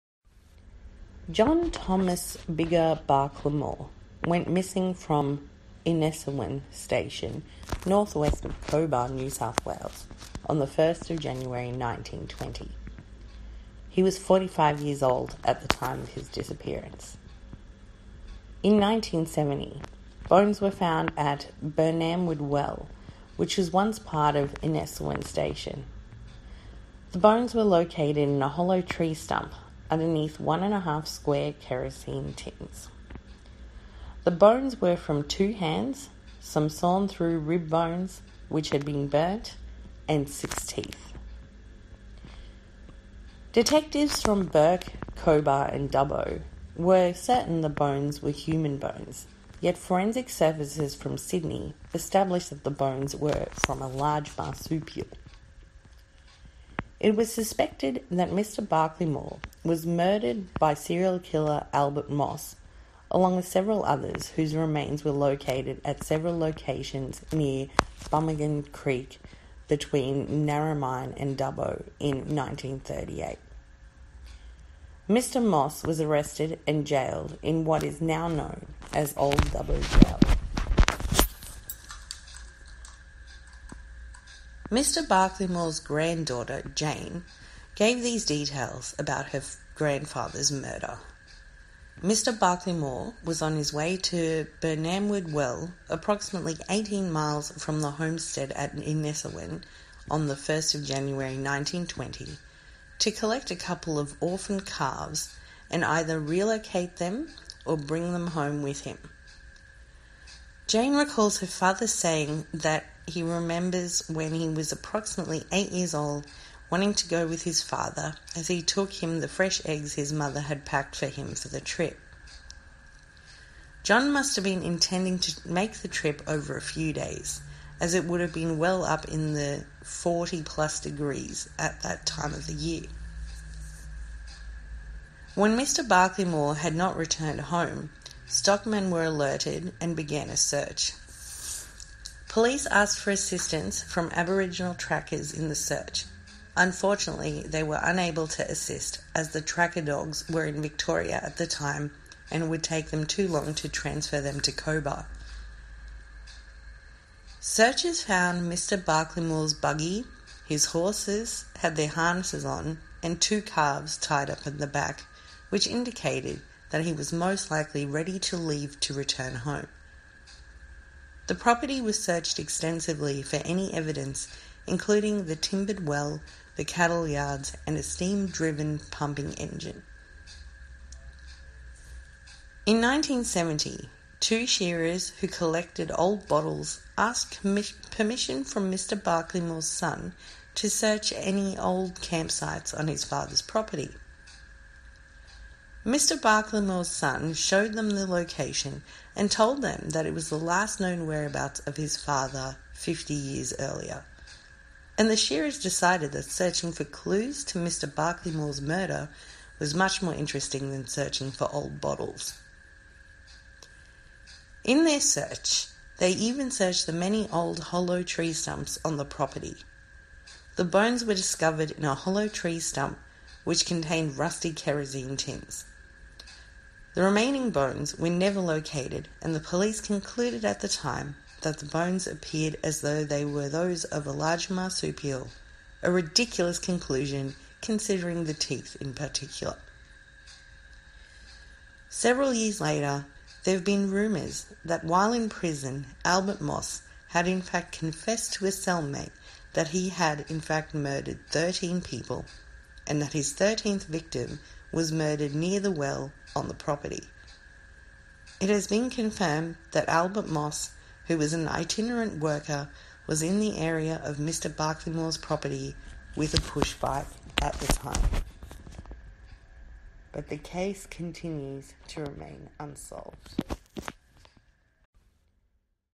MISSING: John Barklimore - Cobar, NSW - 1920
John Thomas Biggar Barklimore went missing from Innesowen Station, north-west of Cobar, New South Wales, Australia on 1 January 1920. In 1970, bones were found at Burnamwood Well, which was once part of Innesowen Station.